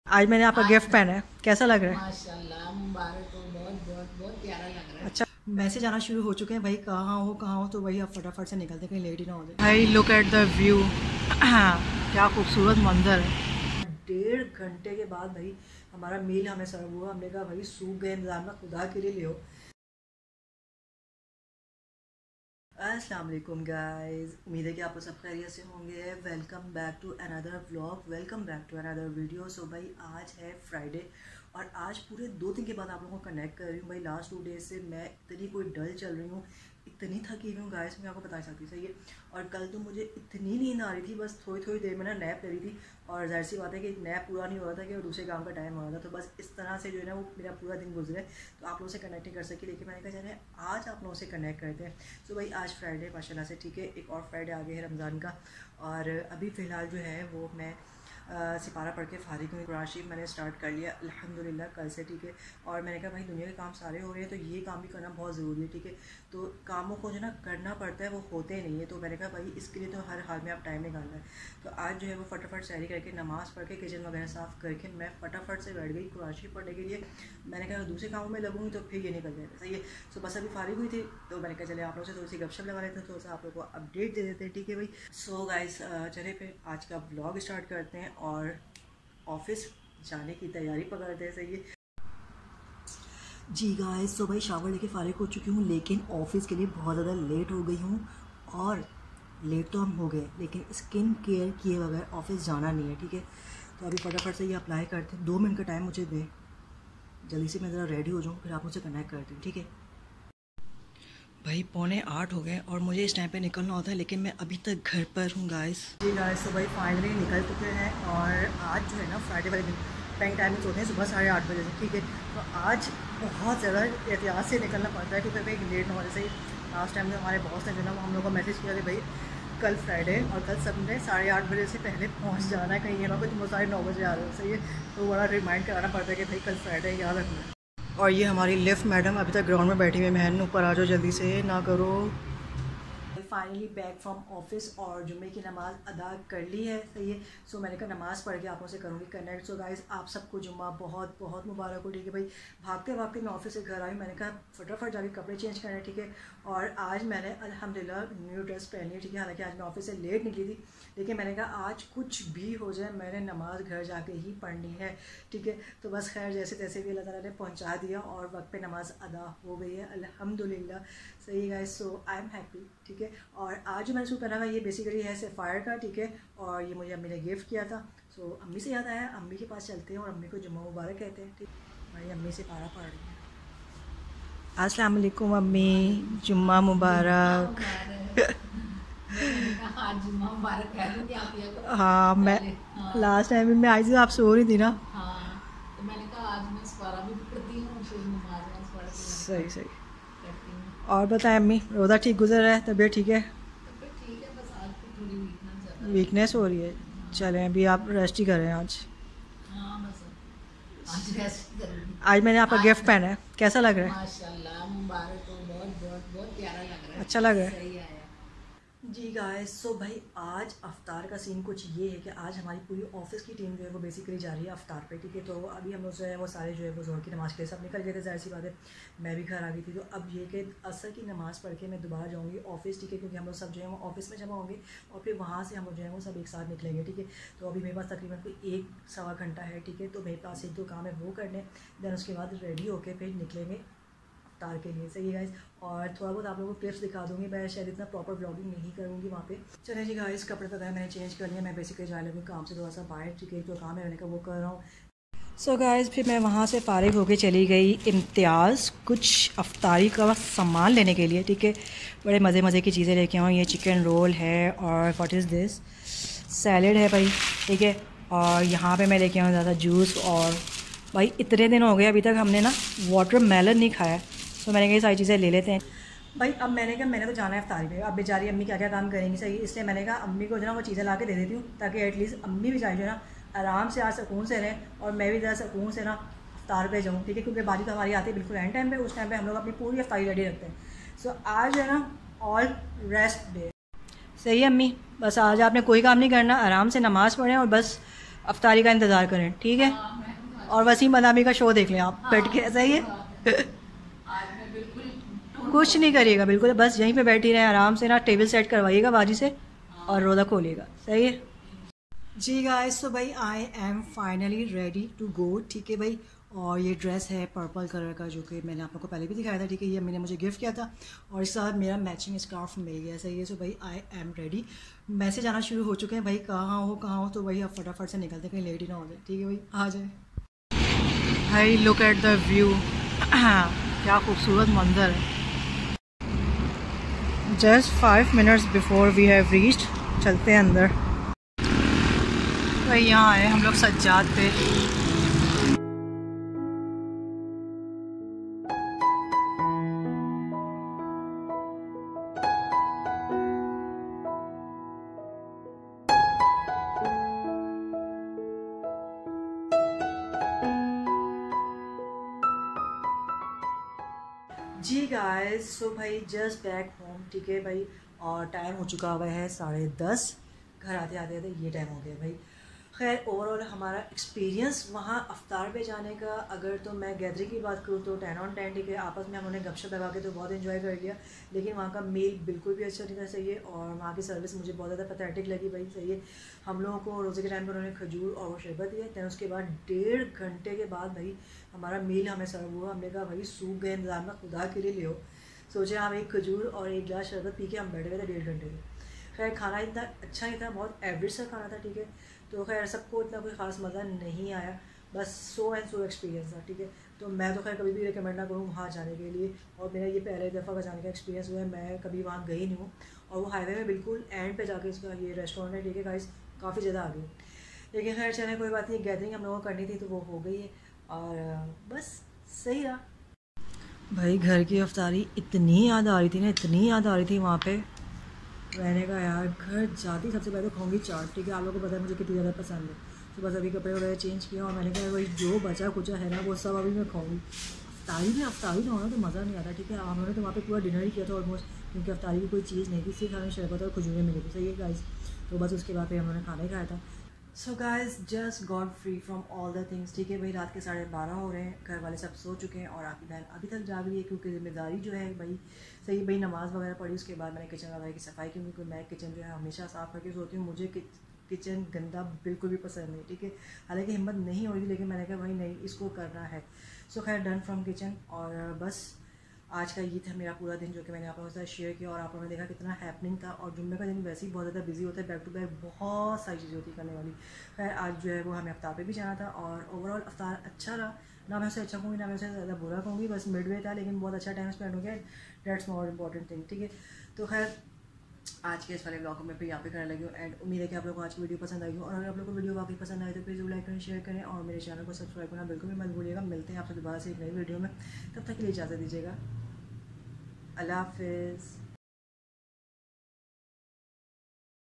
Today's Iftaar with Office colleagues😊| Itna late meal serve hua🙁
Today's Iftaar with Office colleagues. Bohat late meal serve hua.